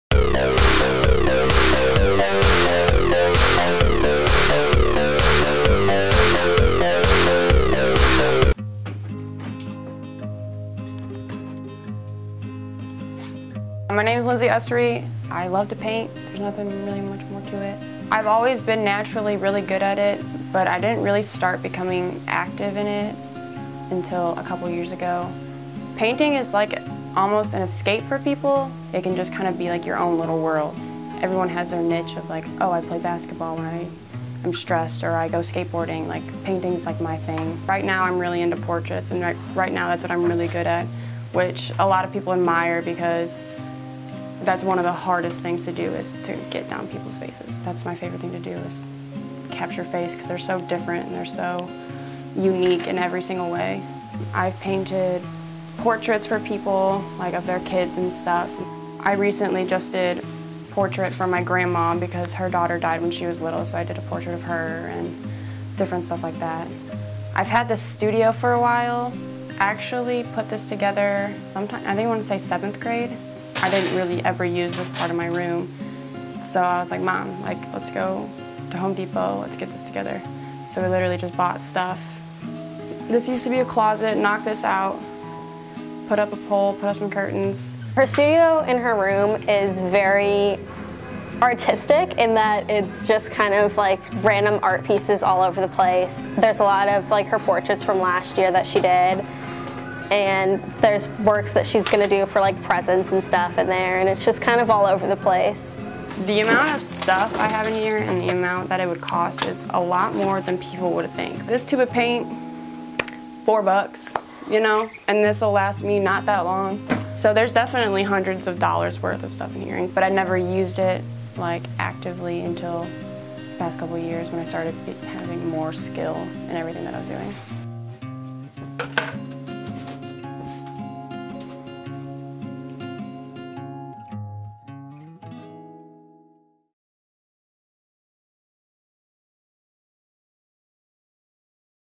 Lindsey Usry's Art Studio
Take a peek inside Lindsey's personal art studio, where she comes to paint and relax.